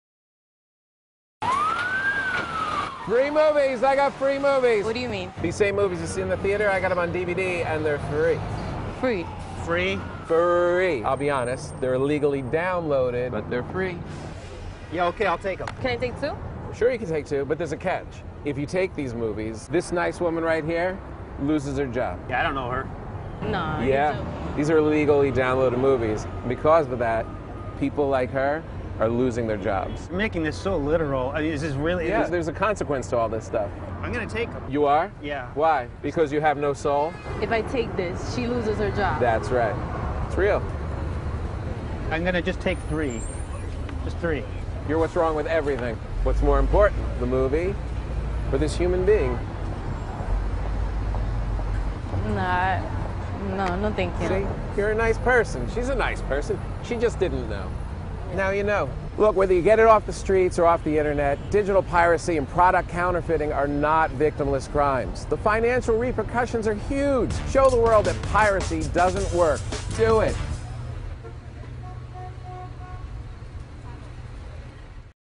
wwwICEgov's Antipiracy PSA - with subtitles
This is a captioned version of Antipiracy PSA video , where you can also translate the captions into other languages.